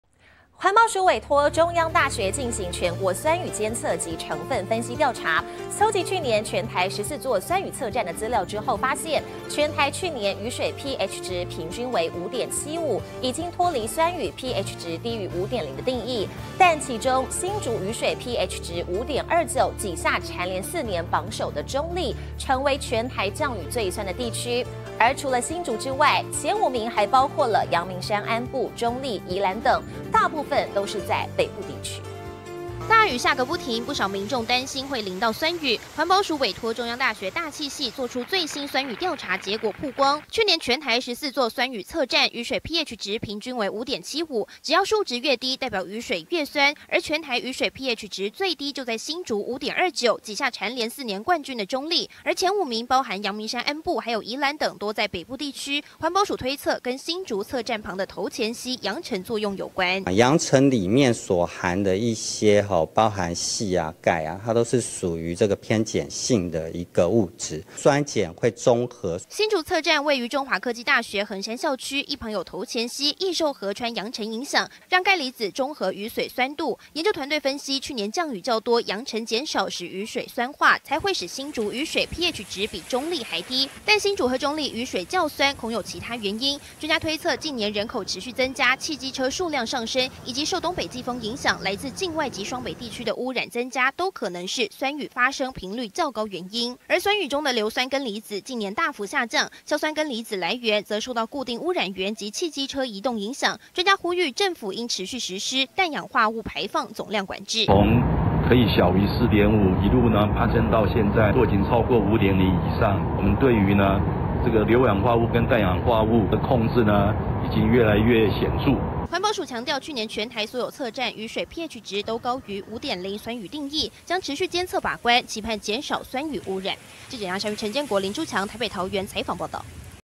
雨水最酸前5名多在北部 中壢第3彭佳嶼第4
環保署委託中央大學進行「全國酸雨監測及成分分析調查」，蒐集去年，全台14座酸雨測站 資料後，發現全台去年 雨水pH值 平均為5.75，已經脫離 酸雨pH值低於5.0的定義，但其中 新竹雨水pH值5.29，"擠下"蟬聯四年榜首的中壢，成為全台降雨最酸的地區。除了新竹之外，前五名 還包括 陽明山鞍部、中壢、宜蘭等，多在 北部地區！